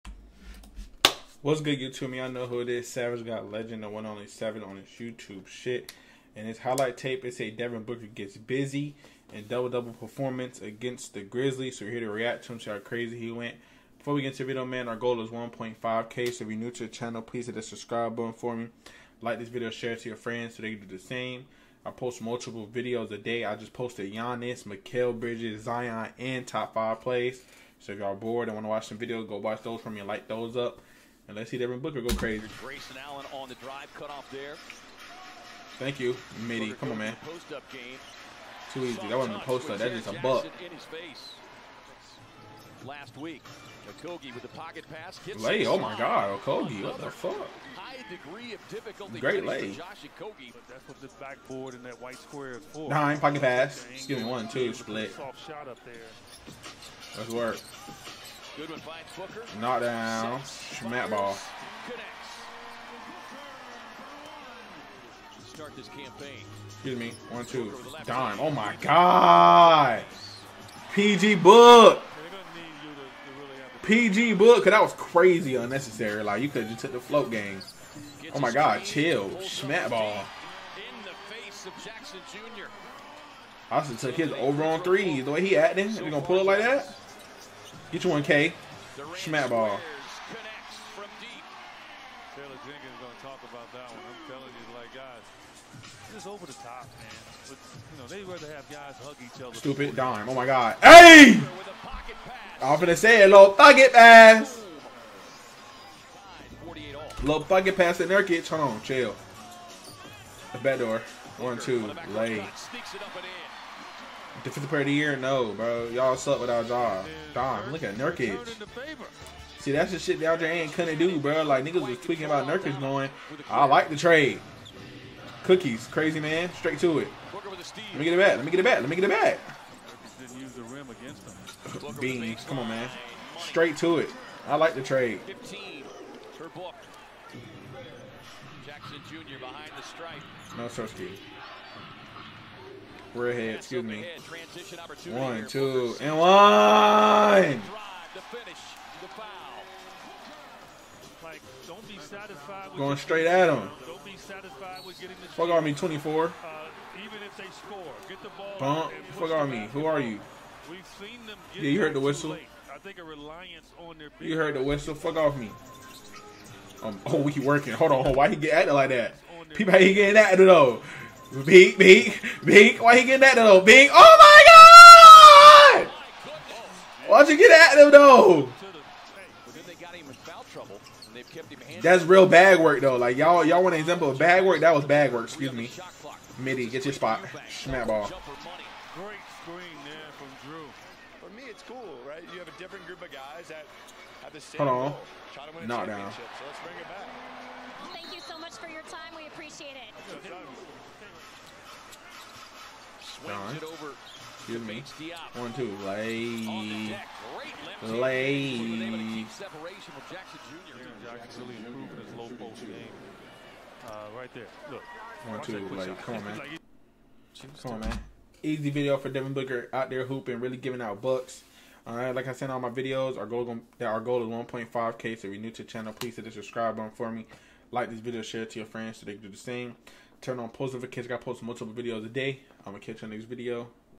Savage Reacts To Devin Booker Gets BUSY In DOUBLE-DOUBLE Performance! ☀ | December 2, 2023
I Like The Trade , Give Them A Little More Time And When Everybody Healthy And They Gel Together They Gon Be Going Crazy.